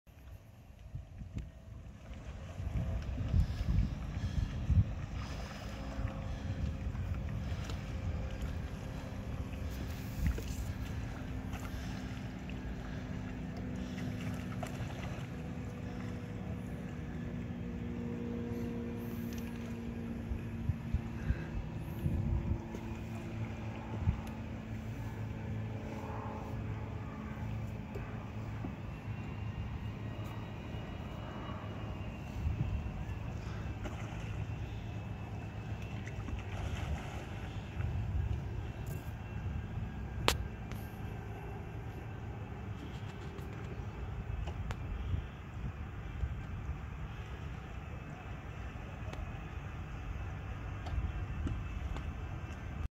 17' Salish Voyager - Test Row
Dave says the new Salish Voyager moves through the water, "Like a razor through velvet." Check out how much glide you can get from a single stroke. Ahhhh.